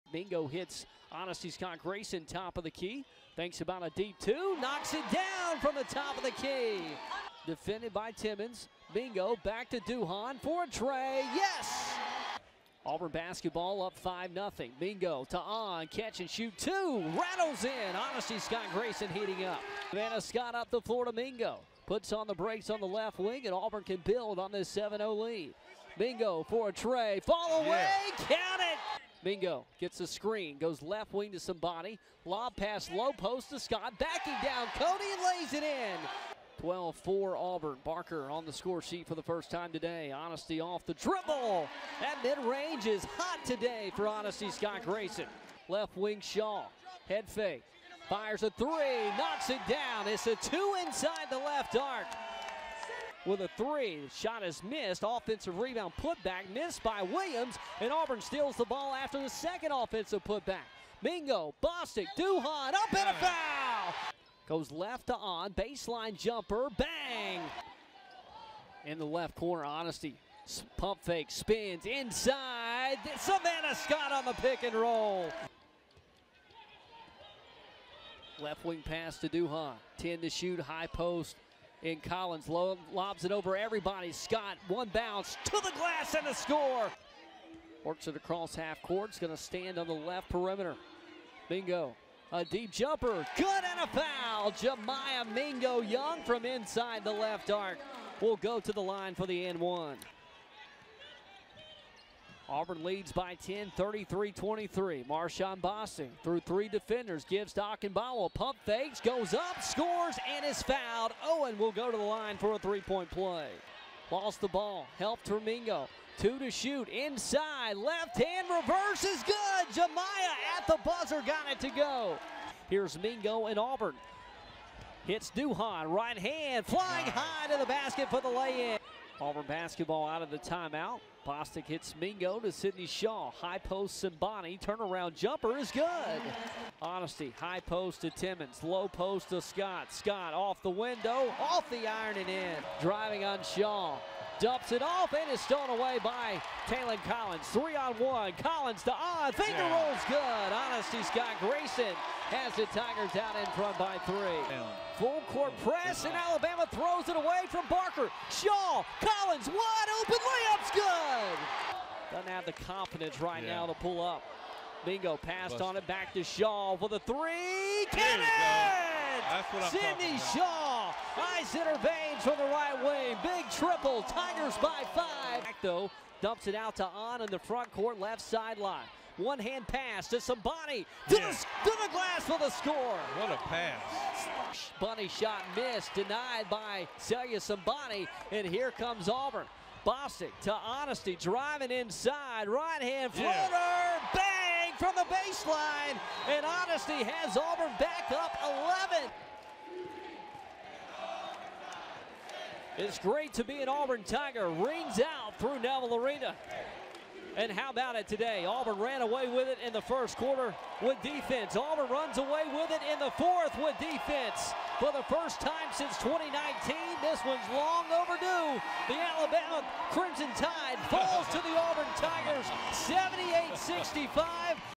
Auburn Women's Basketball - Highlights vs Alabama
On the day Auburn retired the jersey of all-time scoring leader DeWanna Bonner, the Tigers used a stellar start and a fabulous finish to defeat Alabama 78-65 Sunday at Neville Arena.

In the first and fourth quarters, Auburn outscored Alabama 50-20, defeating their in-state rival for the first time in their past 10 meetings.

"Really feels good to get my first win over Alabama, and this team's first win in a while," Auburn coach Johnnie Harris said. "It was special to do this in front of our alumni. We prepared well. Our team was focused, they were locked in. We wanted to set the tone and I felt like we did. Really happy with the win."